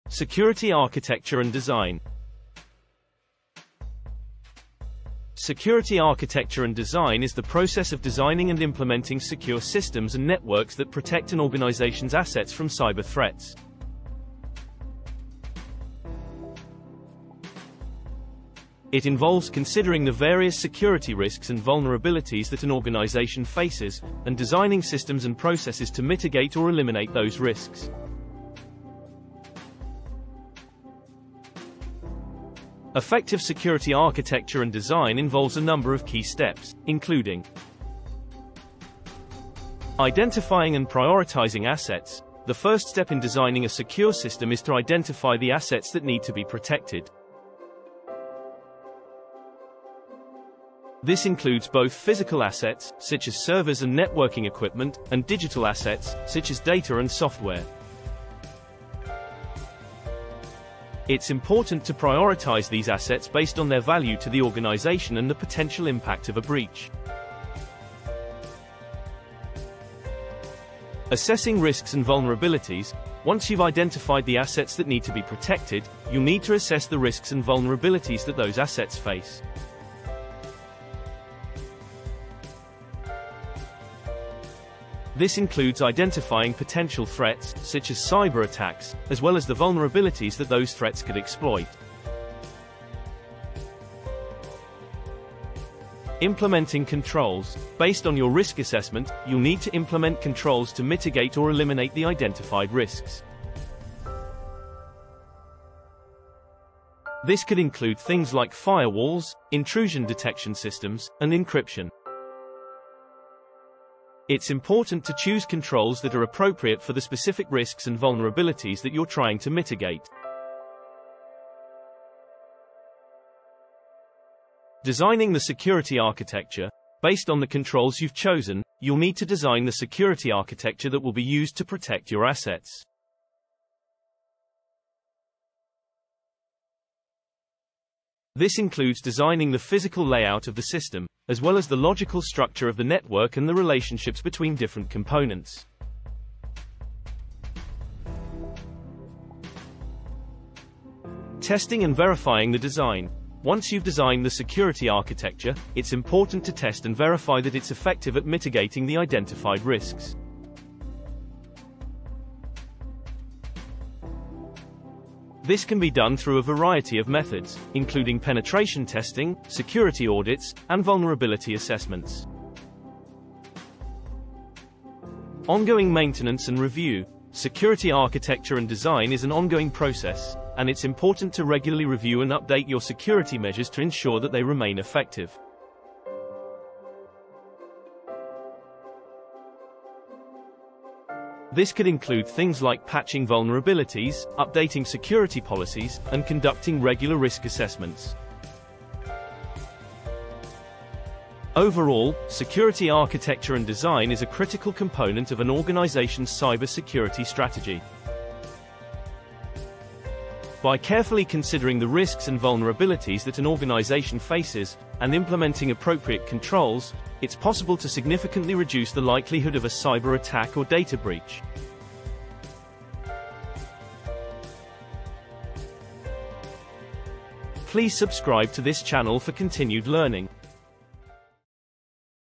Security Architecture and Design
Security architecture and design is a critical component of an organization's cyber security strategy. By carefully considering the risks and vulnerabilities that an organization faces, and implementing appropriate controls, it's possible to significantly reduce the likelihood of a cyber attack or data breach.